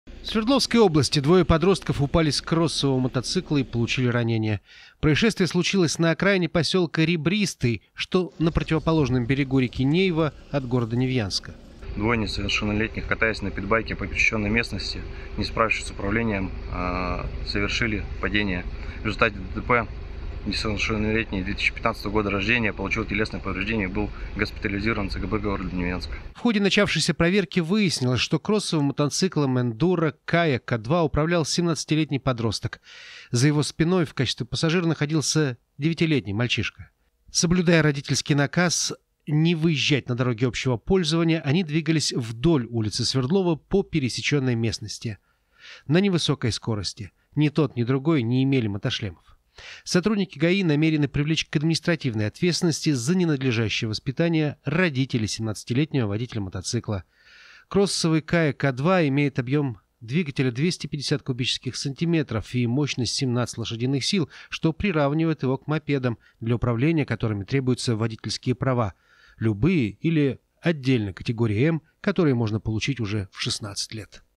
Подростки разбились на эндуро
Под Невьянском 17-летний водитель кроссового мотоцикла и его 9-летний пассажир упали с мотоцикла и получили ранения